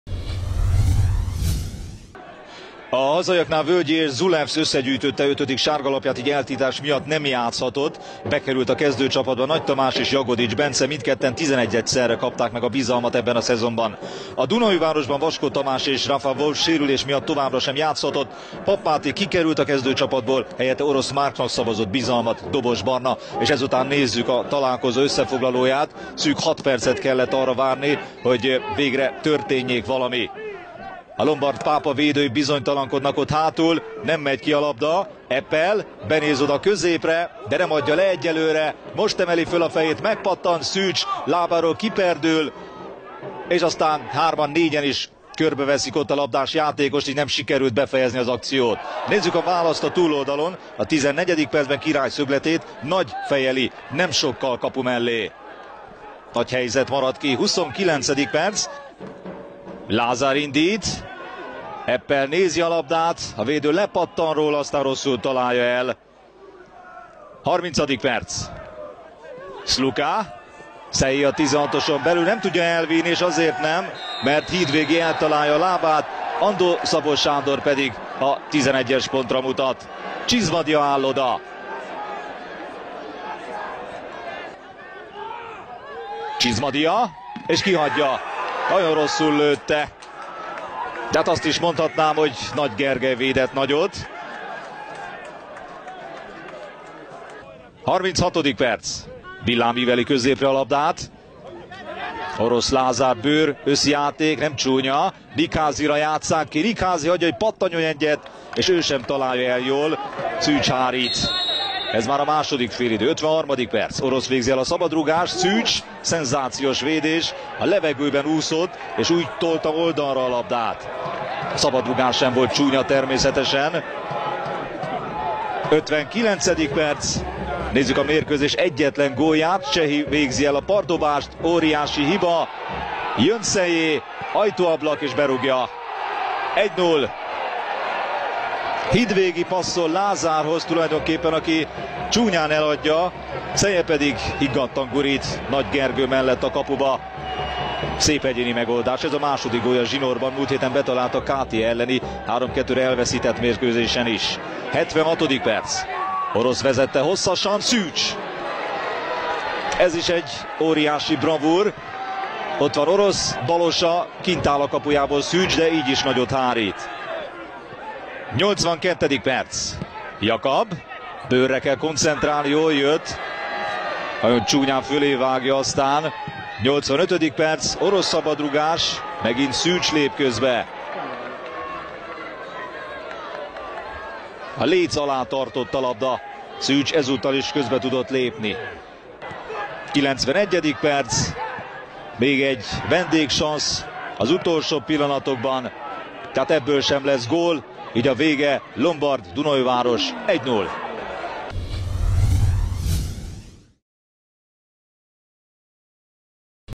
Lombard Pápa Termál FC - Dunaújváros | 1-0 | OTP Bank Liga | 19. forduló | MLSZTV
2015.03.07. OTP Bank Liga, 19. forduló
Lombard Pápa Termál FC - Dunaújváros 1-0 (0-0)
összefoglaló

Játékvezető: Andó-Szabó (Kóbor, Huszár)
Gólszerző(k): Seye (59.)
Sárga lap: Sluka (41.), Popin (90.), ill., Hidvégi (50.)
Kiállítás:

KEZDŐCSAPAT:
Szűcs L.

Csizmadia
Bogunovics
Nagy T.
Frizoni

Stieber A.
Jagodics B.
Pongrácz
Király B.

Seye
Popin
CSEREPAD:
Horváth G. (k)
Eisenberger
Gál Sz.
Illés D.
Sluka
Yannick
Waltner
VEZETŐEDZŐ:
Lengyel Ferenc

KEZDŐCSAPAT:
Nagy G.

Villám
Farkas V.
Hidvégi
Csehi T.

Lázár Zs.

Petneházi
Böőr
Nikházi
Orosz

Eppel
CSEREPAD:
Jahic (k)
Sárközi
Szekeres A.
Jakab
Thiago
Papp M.
Godslove

VEZETŐEDZŐ:
Dobos Barna